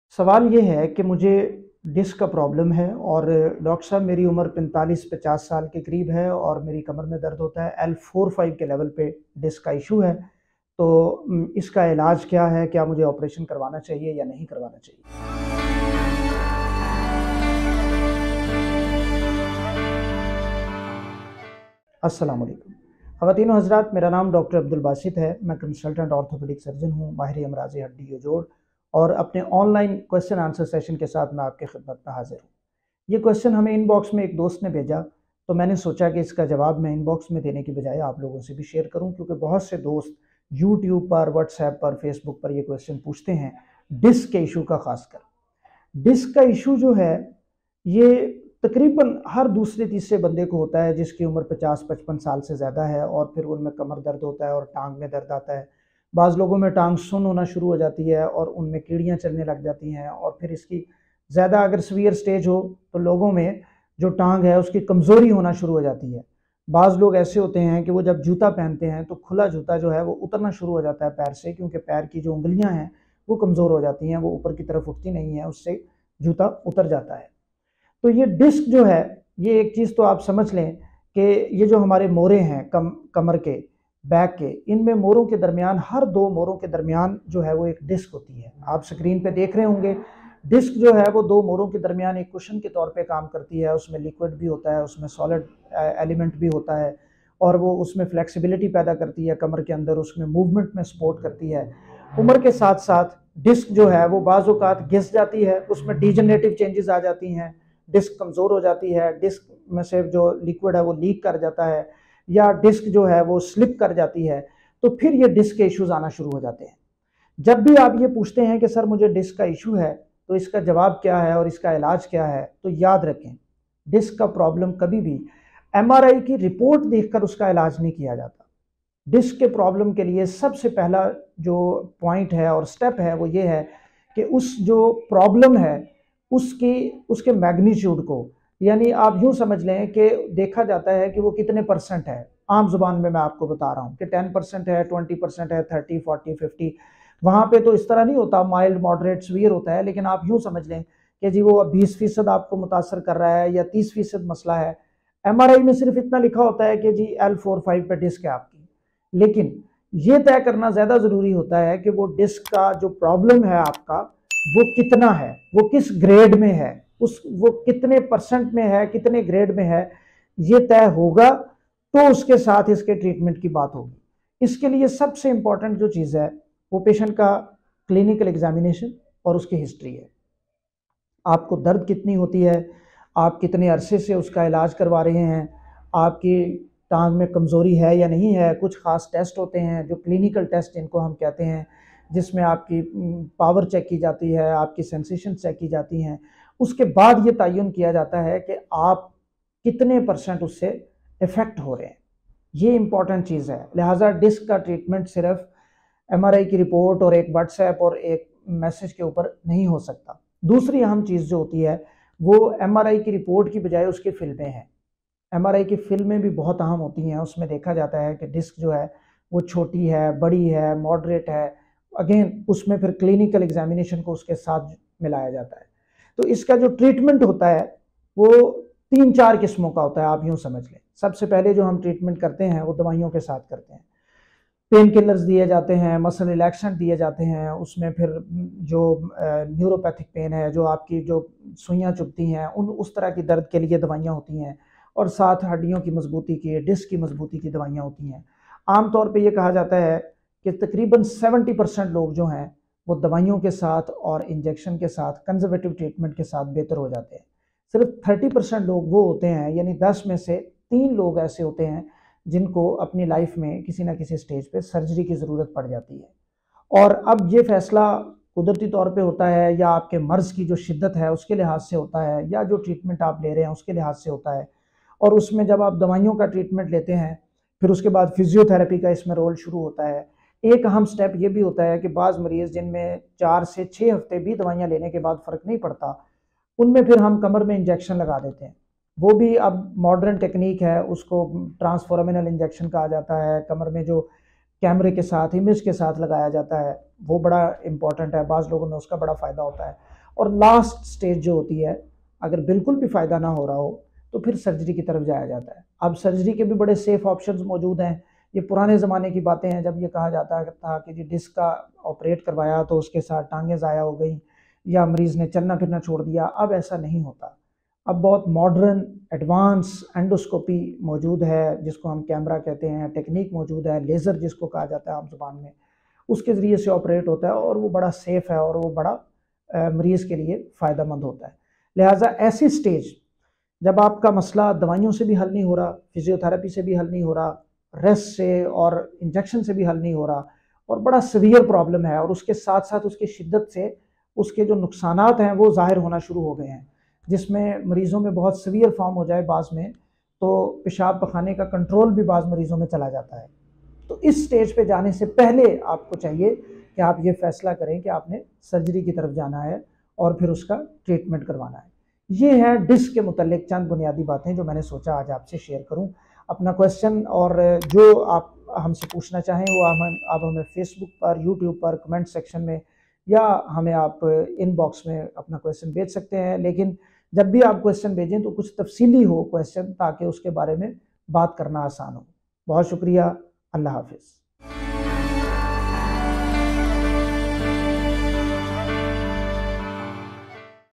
Disc Pain | Disc Problem | Treatment and Exercise for Back Pain | Urdu/Hindi | Dr.Abdul Basit
ڈسک پرابلم
کمر درد ، ٹانگ کا سن ہونا۔۔پرہیز سے علاج تک
کیا آپریشن سے ٹانگیں معذور ہو جاتی ہیں
جانیے کنسلٹنٹ آرتھوپیڈک اینڈ سپائن سرجن ڈاکٹر عبدالباسط سے
Disc Problem | Backache and its treatment | How to avoid Backache | Operation in Low back pain ?
Bone & Joint Clinic with Dr.Abdul Basit Ortho Clinic

Feel free and Share our videos...